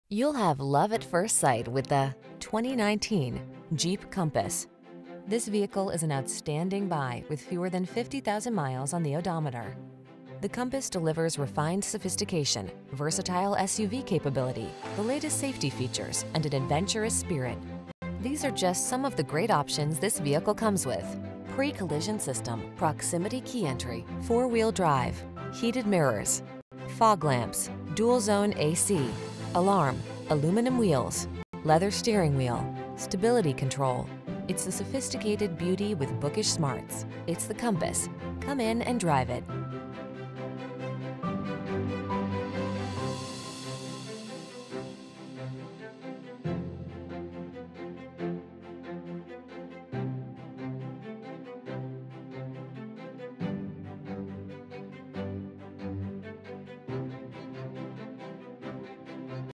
2019 Jeep Compass Antigo, Rhinelander, Shawano, Wausau, Stevens Point 4922A
Billet Silver Used 2019 Jeep Compass Latitude 4x4 available in 123 E Hwy 64, Antigo, Wisconsin at Brickners of Antigo. Servicing the Antigo, Rhinelander, Shawano, Wausau, Stevens Point, WI area.

2019 Jeep Compass Latitude 4x4 - Stock#: 4922A